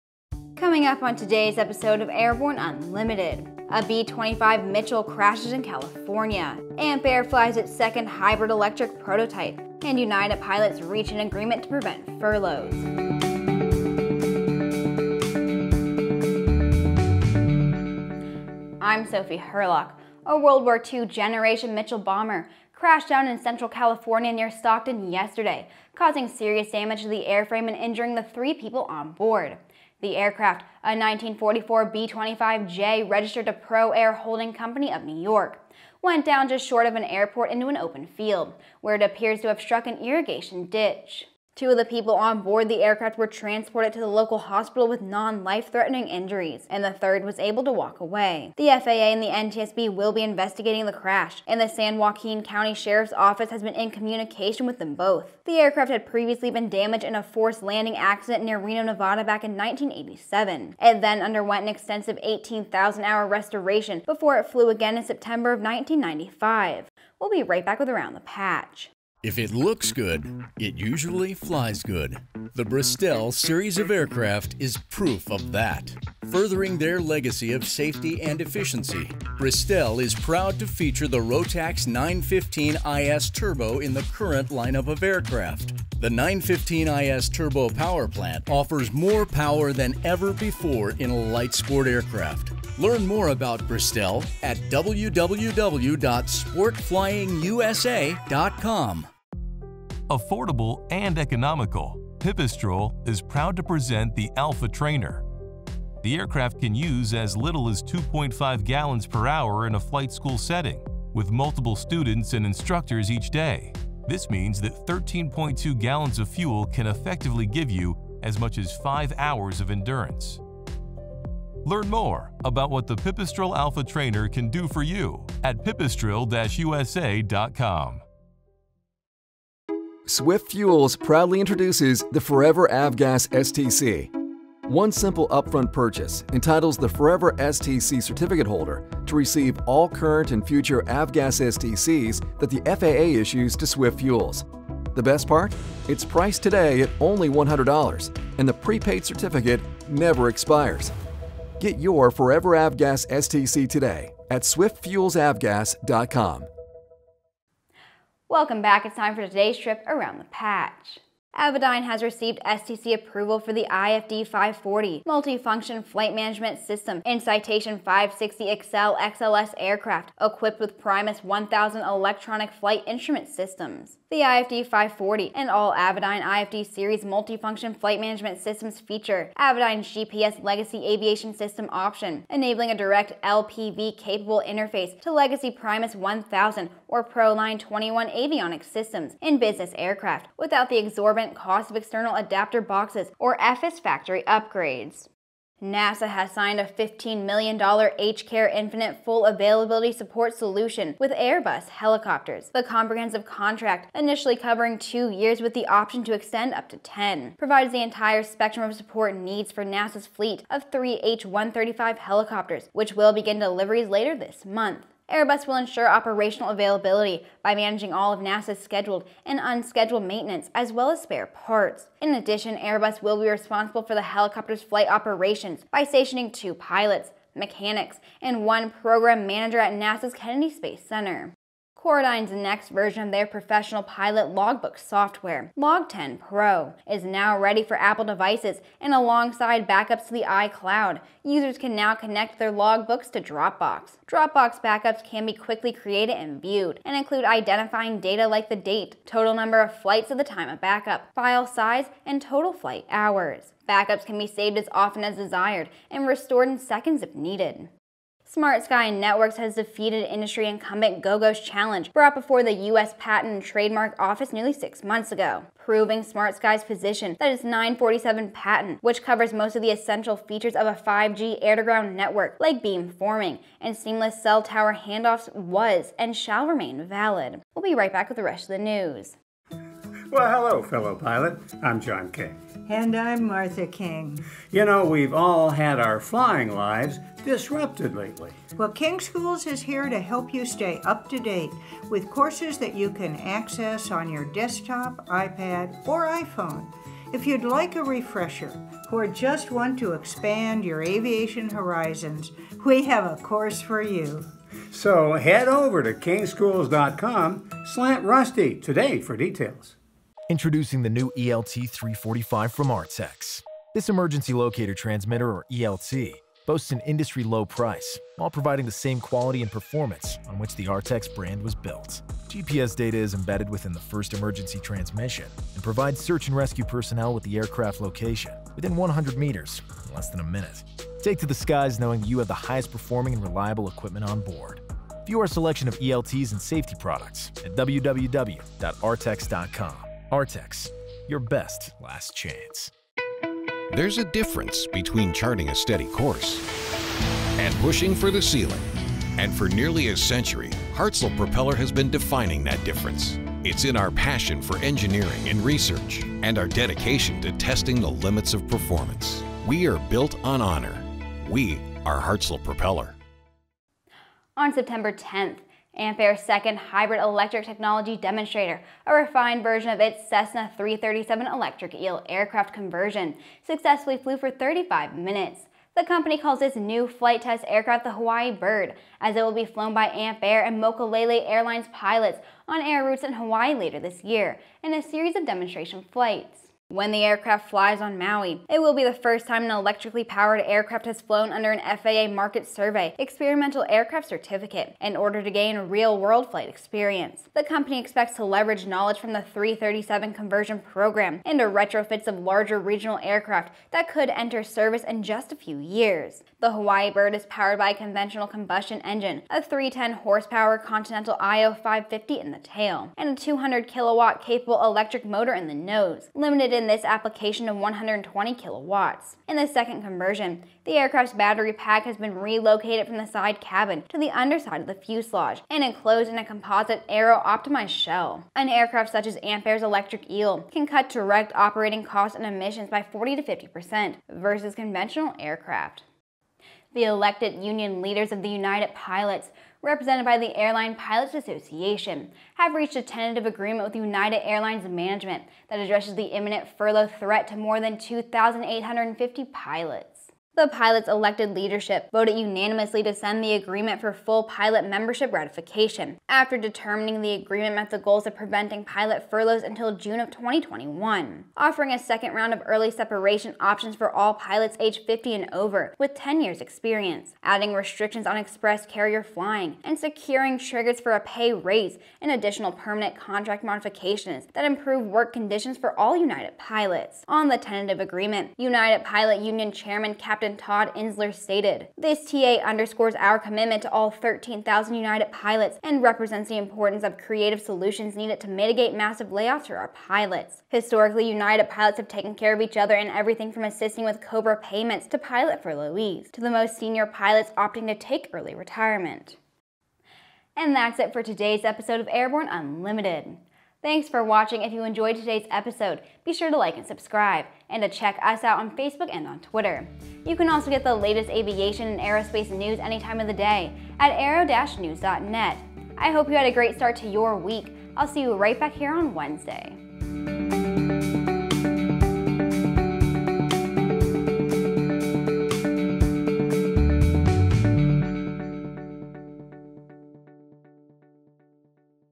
Airborne 09.21.20: B-25 Down In CA, Ampaire Flies Again, UAL Agreement
Also: STC'ed: Avidyne IFD540, Airbus Wins NASA Contract, LogTen Pro Pilot Logbook, SmartSky Beats Patent Challenge

We are still evaluating the information coming in on a crash in the central part of California state, near Stockton,  involving a WWII generation Mitchell Bomber that went down short of an airport and into an 'open' field, where an irrigation ditch, and other impediments, apparently caused serious damage to the airframe.  Three people were on the aircraft. Two were transported to a local hospital, and one was able to walk away. Ampaire has flown its second technology demonstrator. The aircraft is a refined version of its Cessna 337 “Electric EEL” hybrid-electric aircraft conversion. The company refers to the new flight test aircraft as the Hawaii Bird, as it will be flown by Ampaire and Mokulele Airlines pilots on air routes in Hawaii later this year in a series of demonstration flights. When the aircraft flies on Maui, it will be the first time an electrically powered aircraft has flown under an FAA “market survey” experimental aircraft certificate in order to gain real-world flight experience. The elected union leaders of the United pilots, represented by the Air Line Pilots Association, Intl., (ALPA) have reached a tentative agreement with United Airlines management that addresses the imminent furlough threat to more than 2,850 pilots. All this -- and MORE in today's episode of Airborne!!!

Presented by Aero-TV veteran videographer and Airborne Host Sophie Hurlock. Sophie is supported by ANN Chief Videographer Nathan Cremisino, as well as ANN CEO/Editor-In-Chief Jim Campbell.